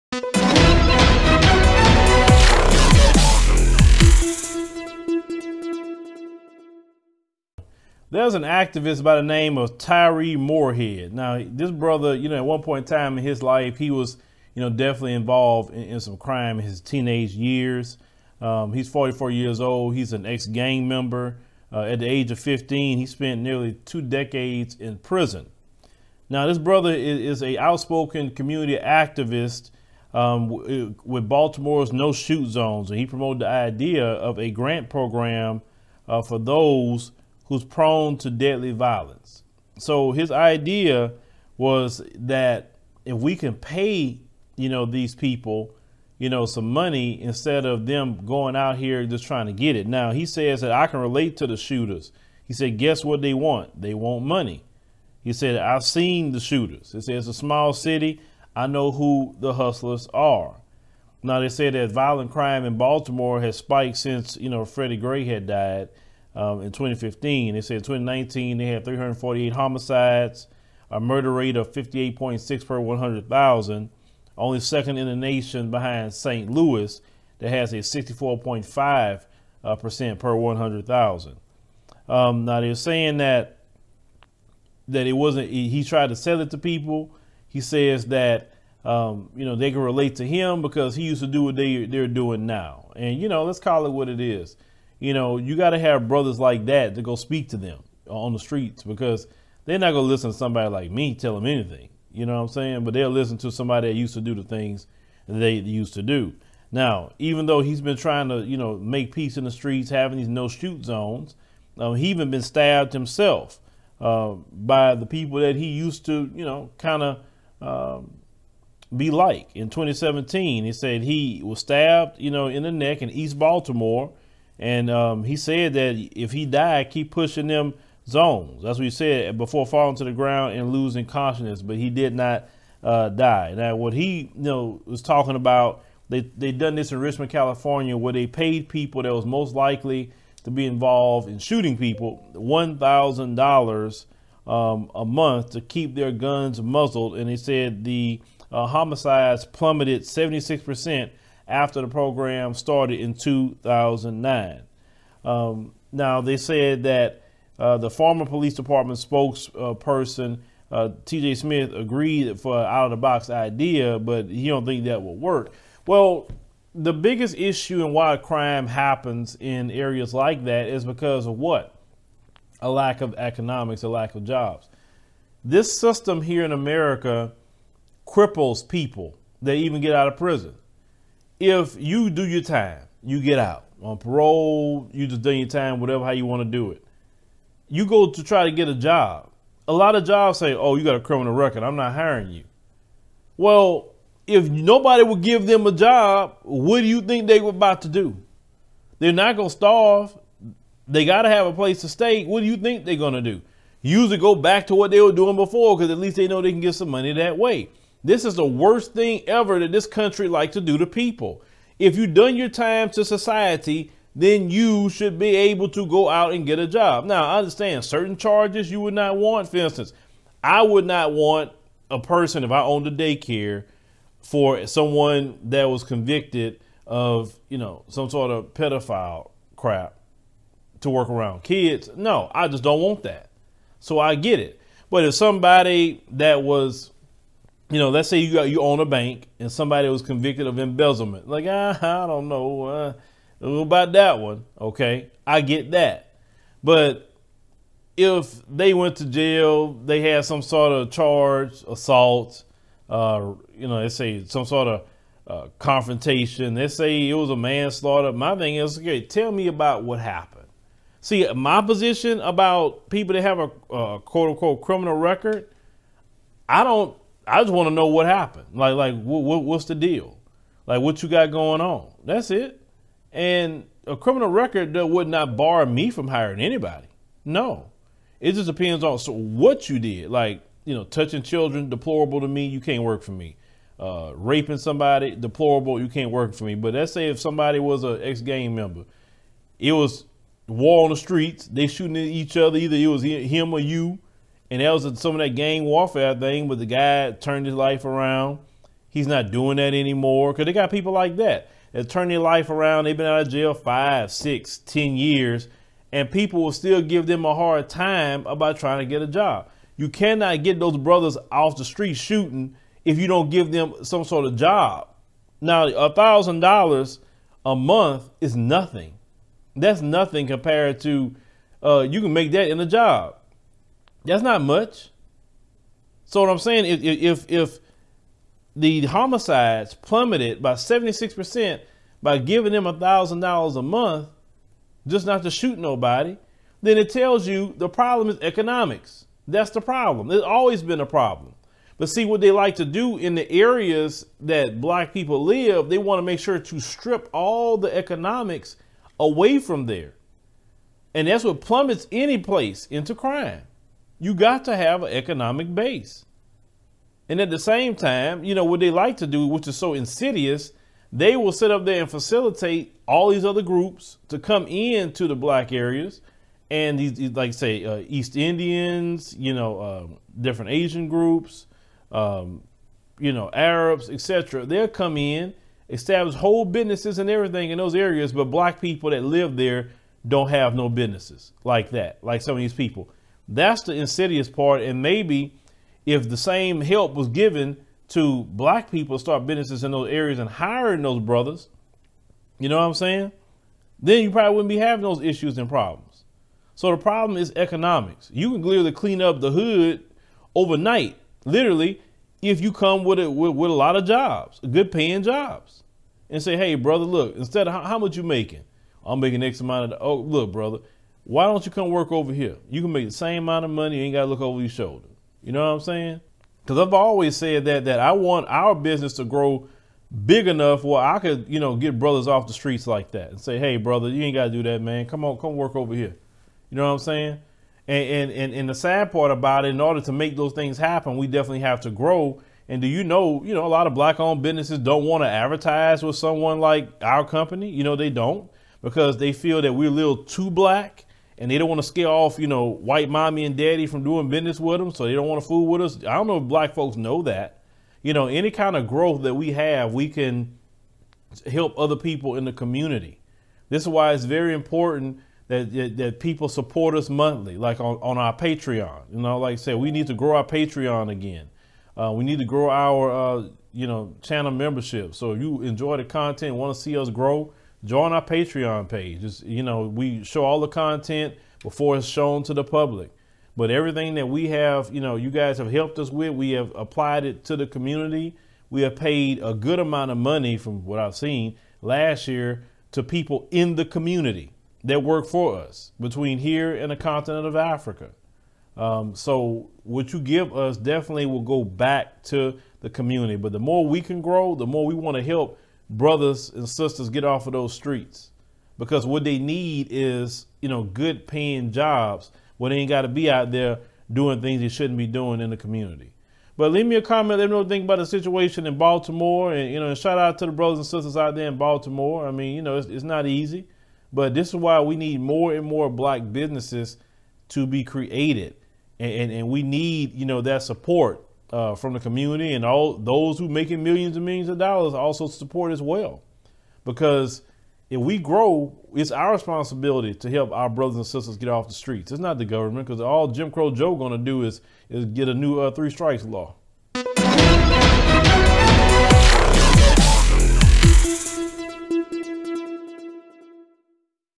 Baltimore Activist Tyree Moorehead Says The City Should Pay Shooters To Stop Doing Crimes
Baltimore Activist Tyree Moorehead wants the city to pay money to people that's committing crimes. Moorehead says the criminals just want money and feels it would be a great incentive.

2323 Clear Lake City Blvd.